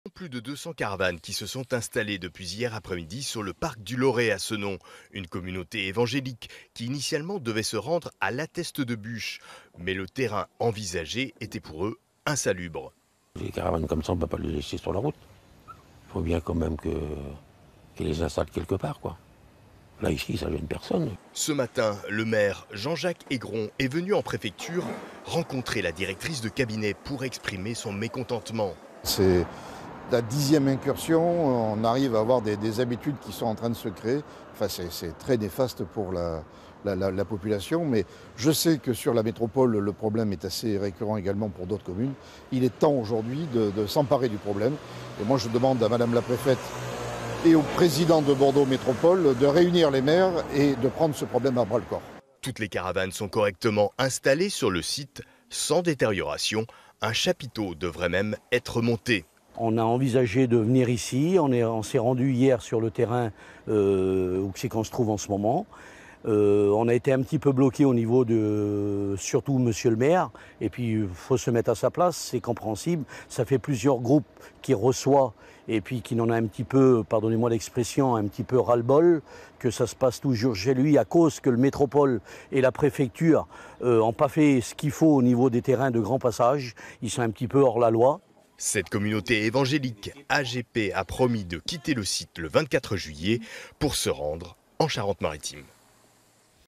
Installation de 200 caravanes à Cenon : le maire ne décolère pas
Au lendemain de l'installation de l'installation de 200 caravanes dans le parc du Loret à Cenon, le maire réclame des solutions face à un problème qu'il qualifie de "récurrent".